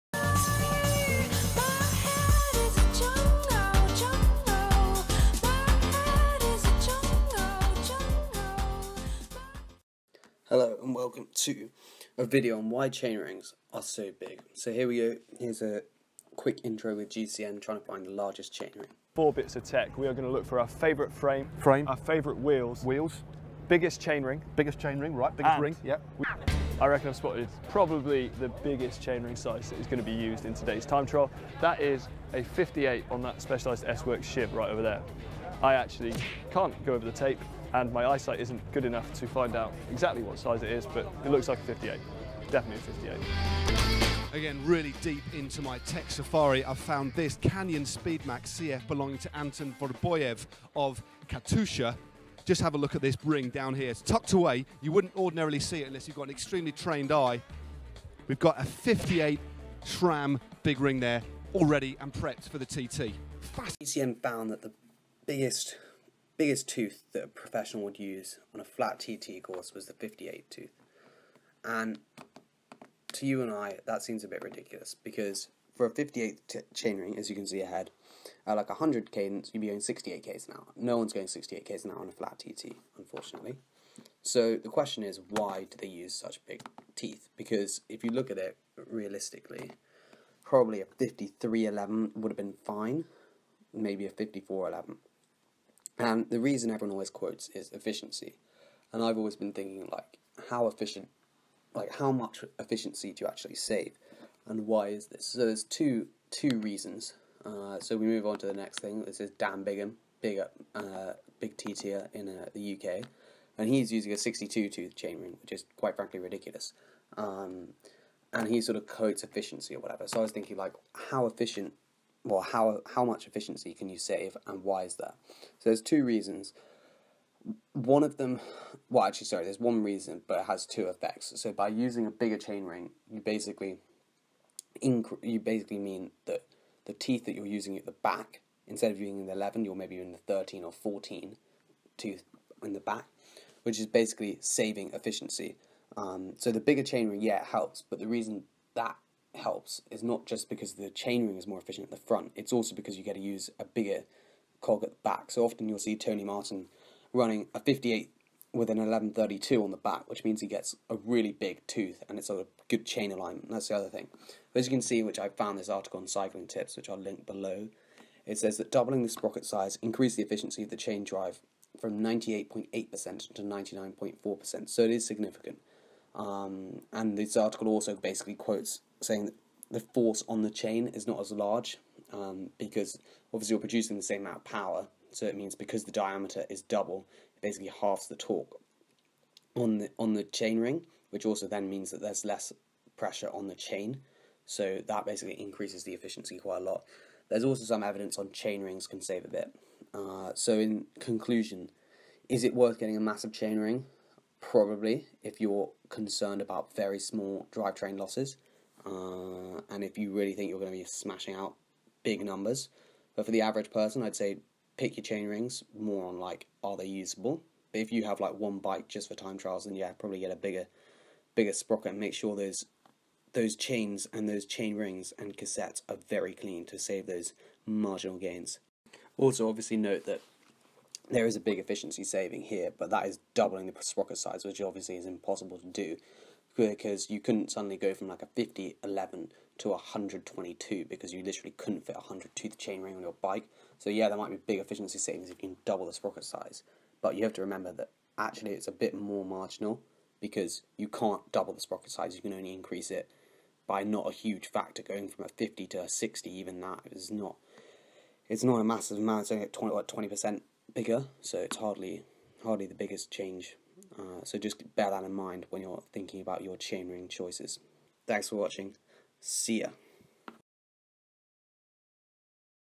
Why do pro's ride such big chain rings?
Are the efficiencies of a big chain ring worth it? I investigate and share my thoughts